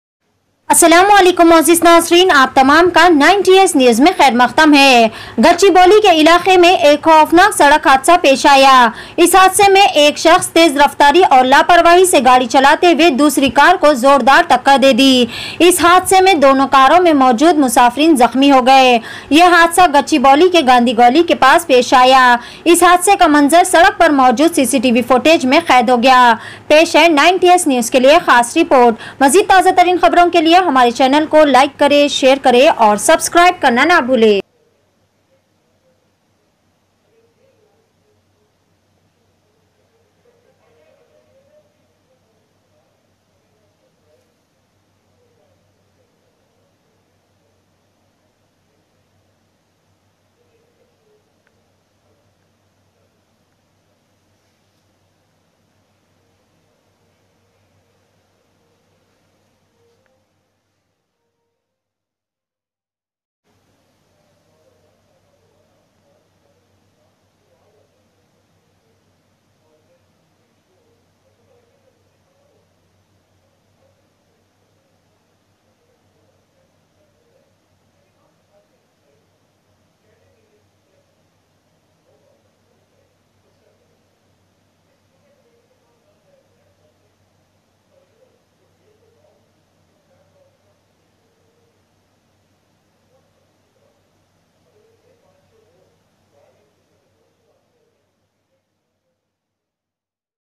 9ts newsCCTV FOOTAGE kless driving at high speed trying to take a turn results in a crash.A n
CCTV FOOTAGE

Reckless driving at high speed trying to take a turn results in a crash.
A non-fatal accident at Gachibowli.